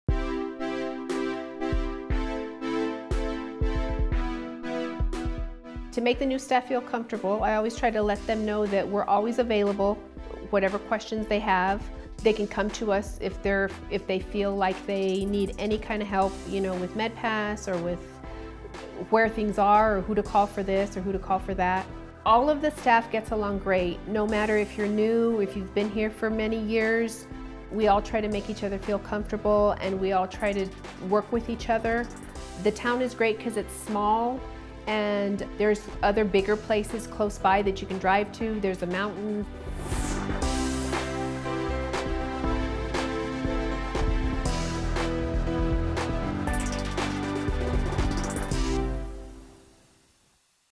Marina Chavez, RN, Describes the Close Working Relationship Between Staff at GCRMC
Marina Chavez, RN, describes the close working relationship between staff at Gerald Champion Regional Medical Center